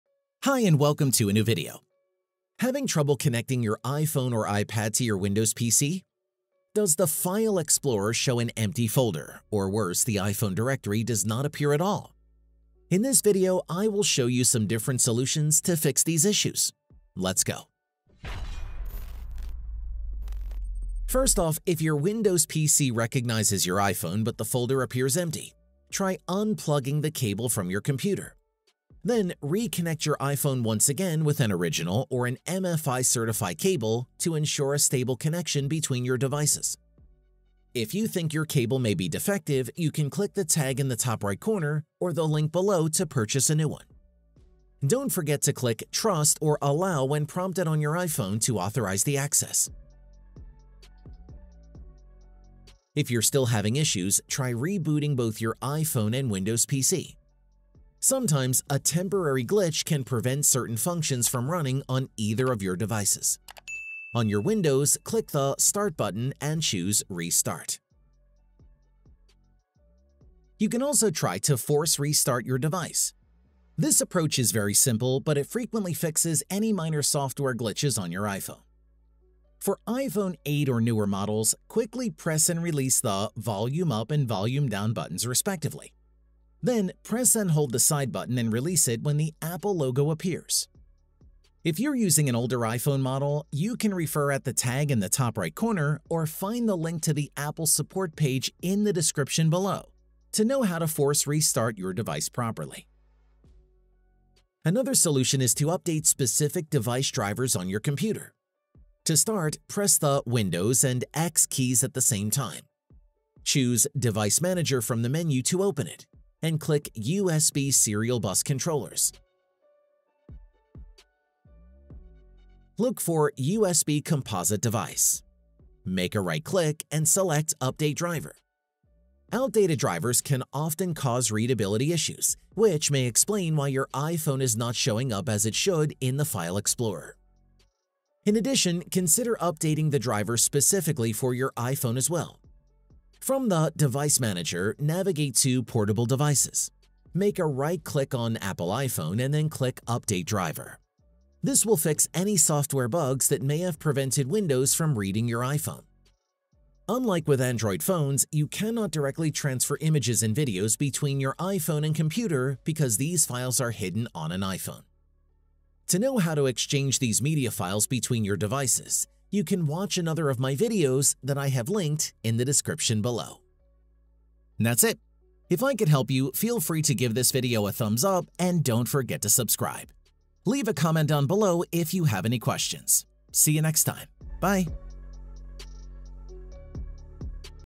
Fix iPhone Not Showing or Empty DCIM Folder on Windows Explorer!
Do you want to transfer or access your media files from your iPhone to your Windows PC, but unfortunately, your iPhone is not showing up on File Explorer? Or does your computer recognize your device, but the folder appears empty? In this video, let's try to resolve this problem with some quick and easy solutions. Yours Jigxor

Most important links:
* | USB-C Lightning Cable
* | USB Lightning Cable
| Transfer Media From iPhone to Windows

Timestamps:
00:00 | Re-plugin Device
00:52 | Restart Devices
01:43 | Device Drivers
02:36 | Exchange Images/Videos

Video: 440